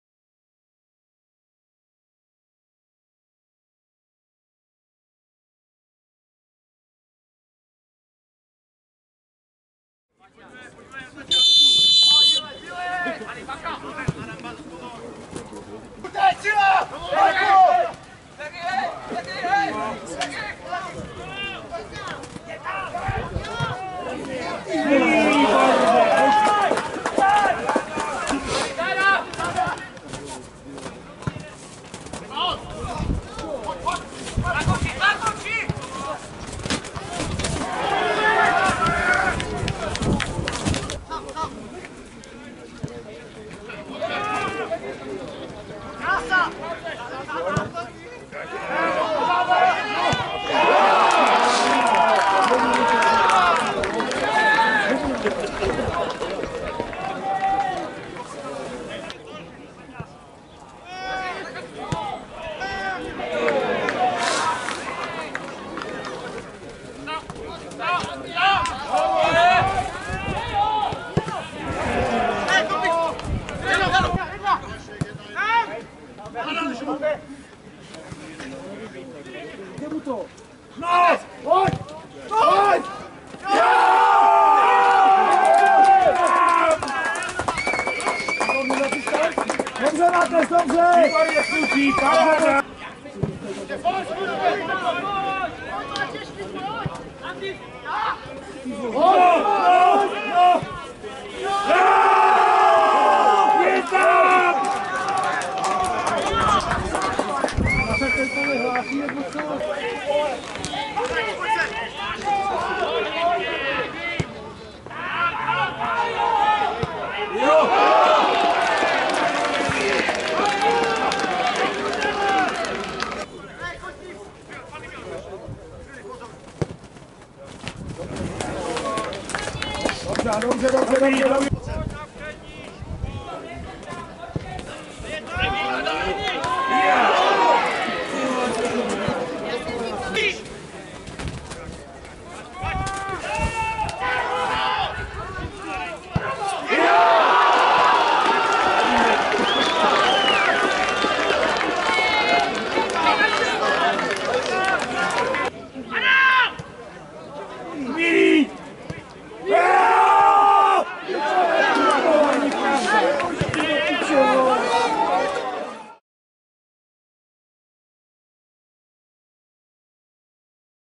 Ústí - Bohuňovice 3:2 (1:0)
Hráči Ústí se radovali z gólu v samotném závěru prvního poločasu proti Bohuňovicím, když se trefil Patrik Sencovici. Hostující tým o dvacet minut později slavil hned dvakrát a slepenými zásahy během minuty otočil vývoj utkání. To ale ještě nebyl konec, Sencovici o chvíli později srovnal a poslední slovo měli opět domácí. V 87. minutě poslal míč do bohuňovické sítě Frydrych a Ústí se stále drží na dostřel první příčky PB SCOM krajského přeboru.